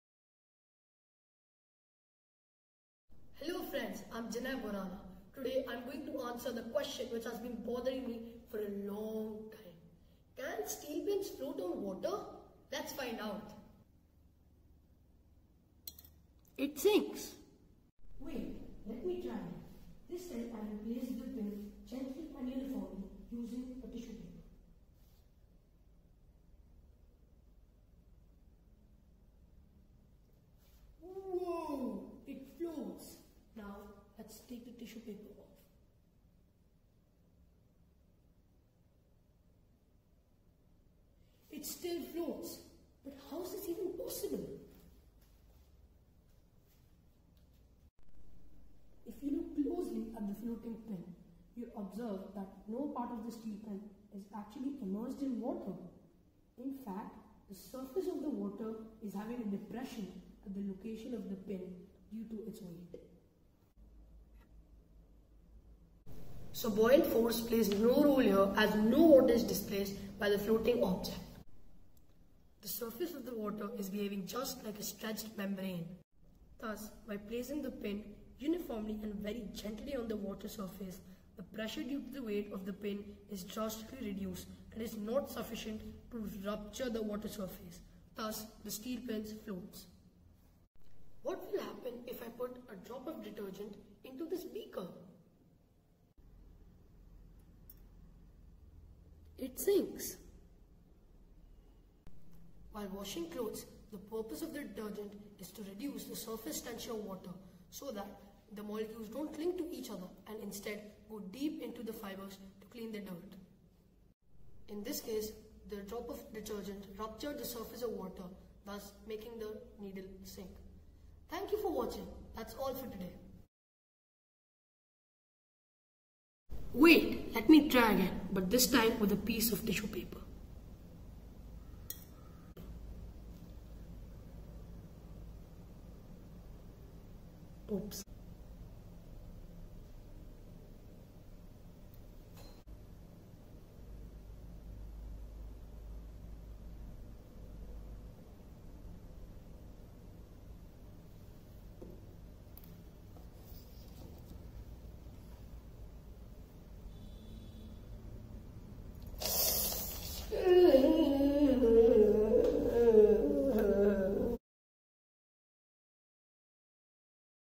How can steel pins float on water?
Jinay explains how it is possible to make steel pins float on water even though steel is much denser than water. The explanation is not the same as how steel ships can float on water, since no part of the steel pin is actually under water, and so the steel pin does not displace any water. So no buoyant force or upthrust is produced here, since no water is being displaced by the steel pin. The weight of the steel pin is actually supported by the surface tension in the water surface due to which it behaves like a stretched membrane. It can support weights as long as the pressure due to the weight is insufficient to rupture the membrane.